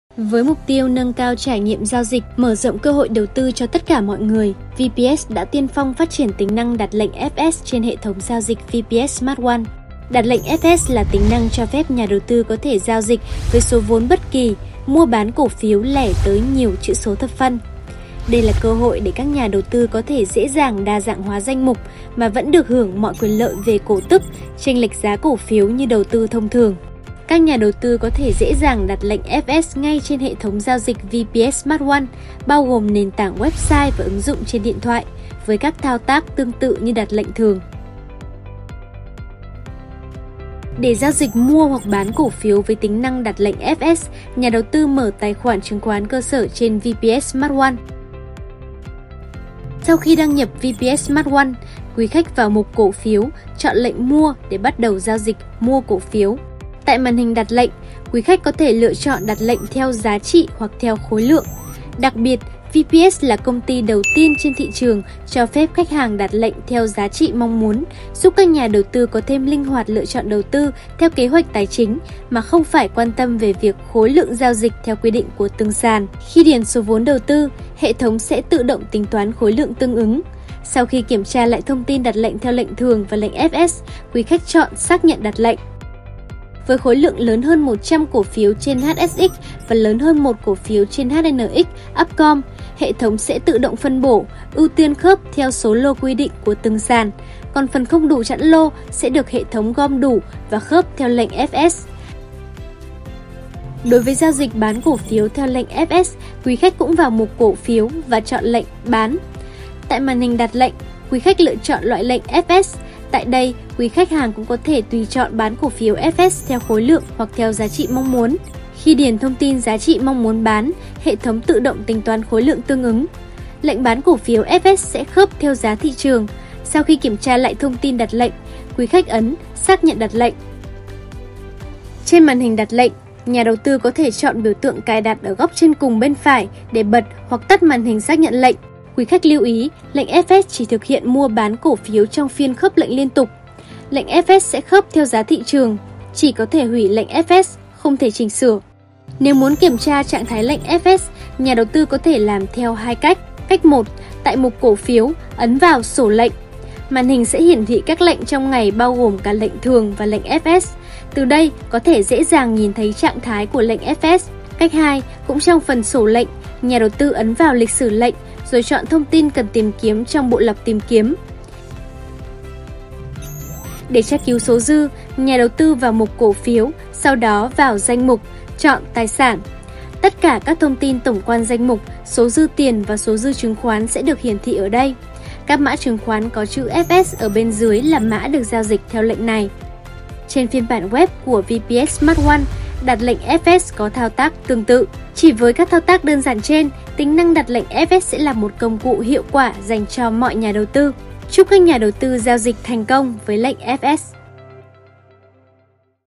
Hướng dẫn đặt lệnh FS trên hệ thống giao dịch VPS SmartOne
Nhà đầu tư có thể truy cập hệ thống giao dịch VPS SmartOne và trải nghiệm tính năng đặt lệnh FS đột phá 💻 📱
Với FS, nhà đầu tư có thể:
👉  Tham gia đầu tư dễ dàng với số vốn bất kỳ
👉  Mua/Bán cổ phiếu tới nhiều chữ số thập phân
👉  Đa dạng hóa danh mục đầu tư với mọi mã cổ phiếu
Đặc biệt, với tính năng đặt lệnh mua/bán theo giá trị, nhà đầu tư chỉ cần nhập số tiền mua/bán mong muốn, hệ thống sẽ tự động tính toán và hiển thị số lượng cổ phiếu cần giao dịch. Từ đó đạt được dự do trong quyết định giao dịch, linh hoạt trong kế hoạch đầu tư và tối ưu hoá trải nghiệm đặt lệnh ✔
⏩ Truy cập hoặc tải ứng dụng phiên bản mới nhất để trải nghiệm tính năng xịn xò nhé
💡 Nhà đầu tưcó thể xem Huong_Dan_Dat_Lenh_FS hướng dẫn siêu đơn giản, siêu chi tiết trong video hoặc tham khảo HDSD bản mềm tại đây